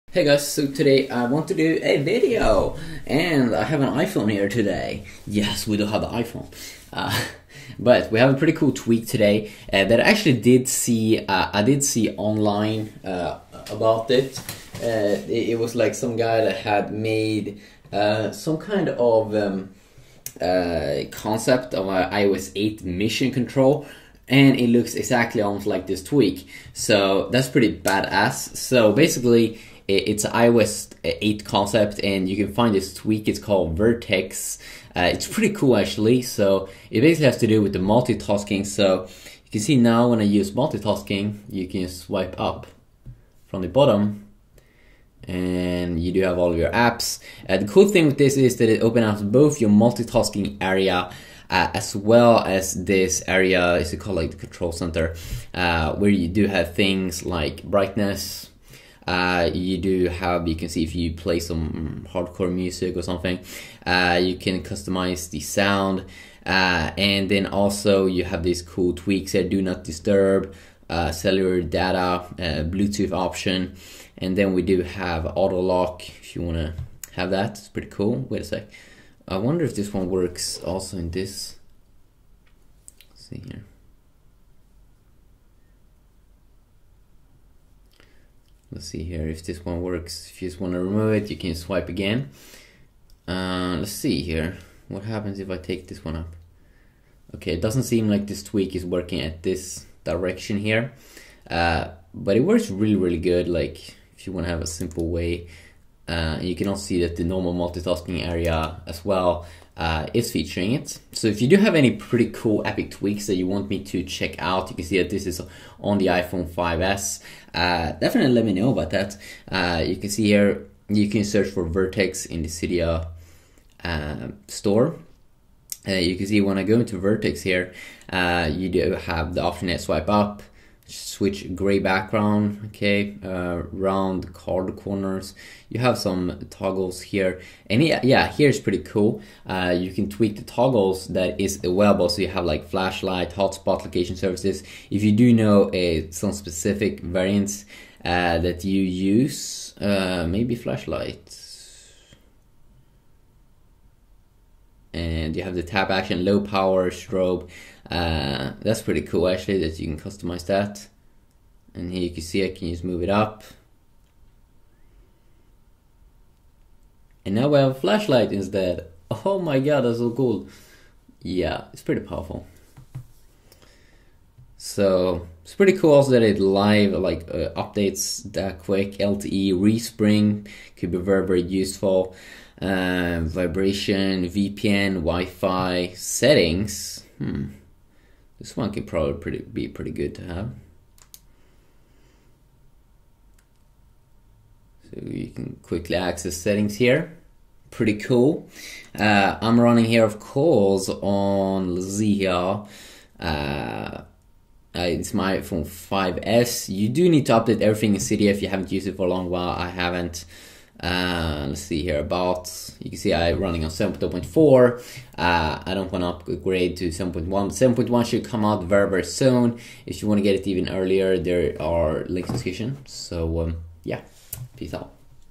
NEW iPhone 5S iOS 8 Mission Control Concept Tweak 2014
New cool cydia tweak called Vertex combines control center + multitasking inside iOS.